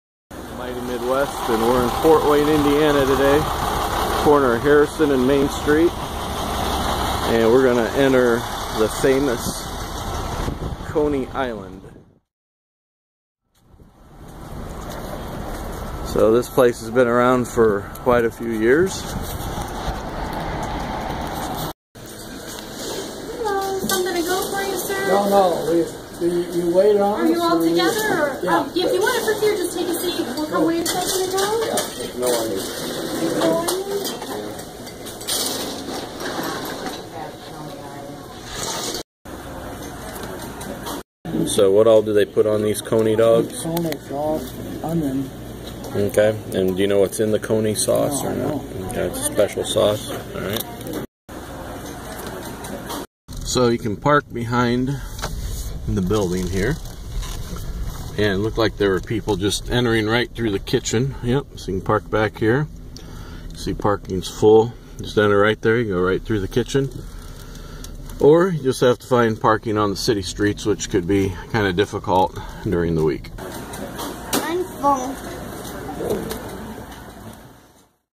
Coney Island's "secret sauce"
at Fort Wayne's Famous Coney Island it's all about the sauce.
 Coney Island has been serving it's famous Coney Dogs for over 100 years now.
  I got mine Coney Style ( with sauce & onions ) & I added cheese. They also have chili dogs, soups, &  hamburgers. The coney dogs were only 1.50 a piece.
  Not even the workers know the secret ingredients to the specialty sauce
 Worth the trip for the nostalgia